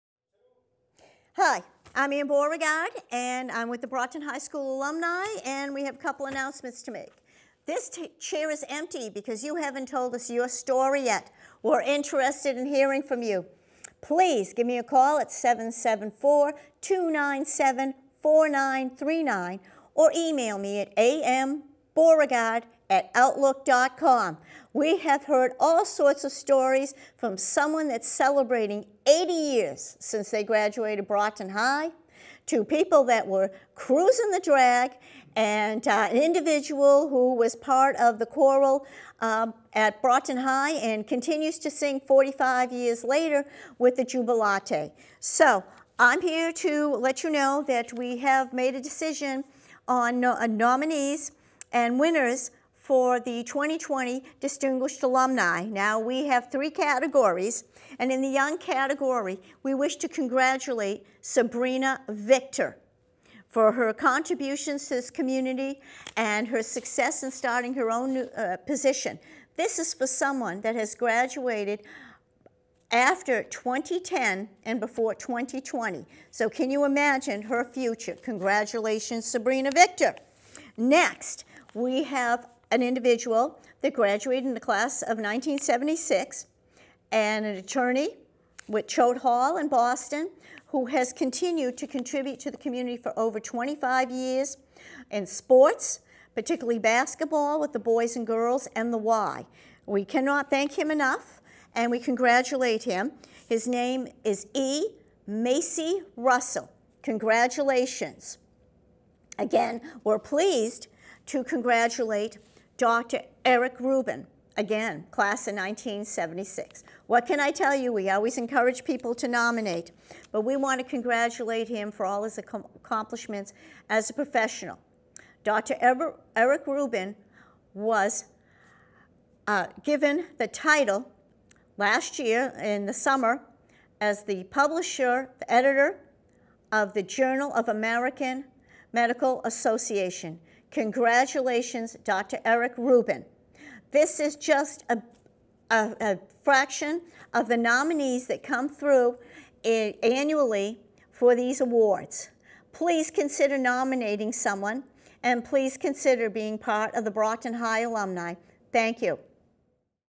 Brockton High School Alumni Association PSA
Anne Beauregard, president of the Brockton High School Alumni Association, reports the winners of the Distinguished Alumni Awards.